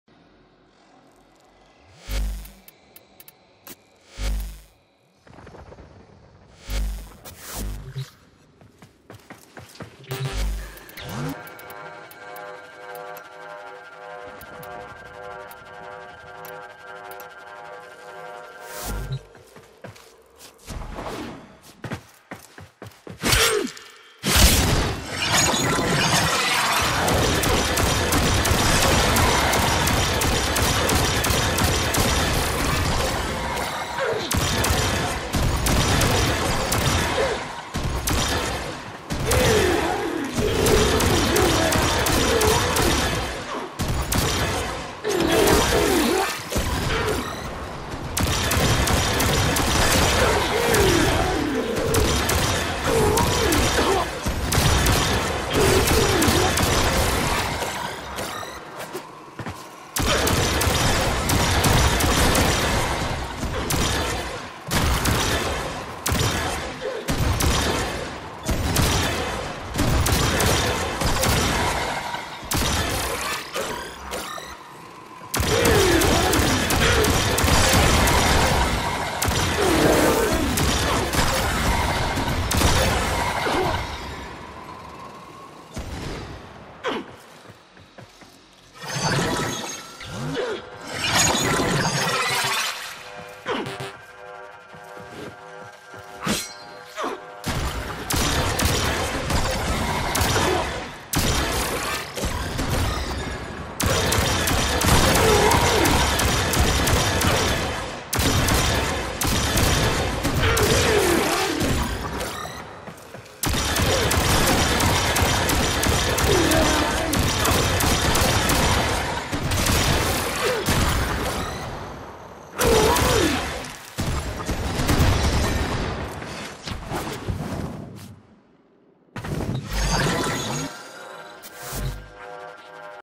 Simulor performance for that Reddit guy who asked
Also, the time it stopped firing were a bug. I was pressing LMB through the whole time, spamming it or holding didn't help. I'm using this video to report the bug as well.